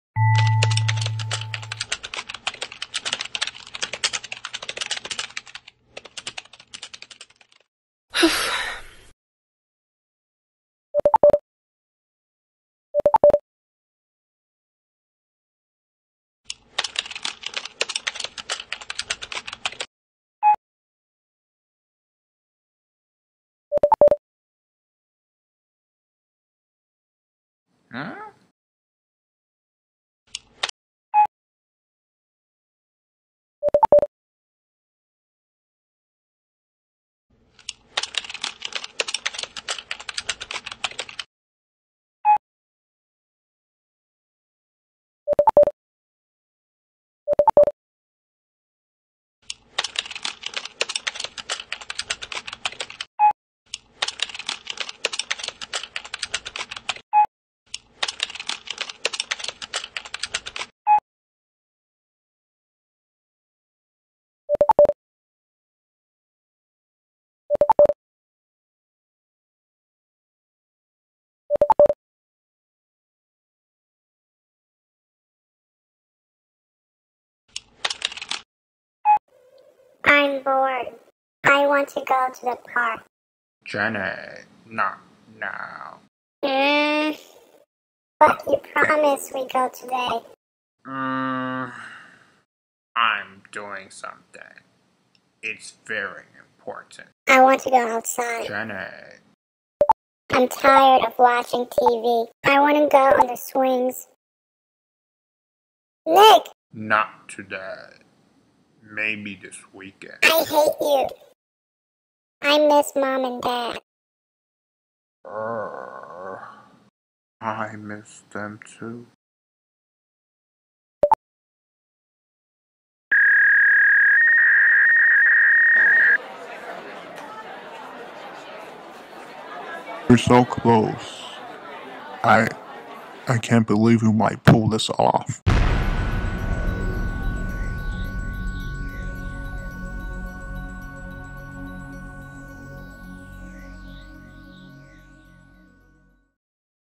Enter Sumo 1.1 (My little sister is so ANNOYING!)
Nick and Jenny at home apartment.

The first episode and scene of the Enter Sumo cartoon series. Powered by the Blender open-source 3D computer graphics software and assembled through the iMovie application.

Typing sound from "Sounds of Typing" by Michael Mol (Attribution 3.0 United States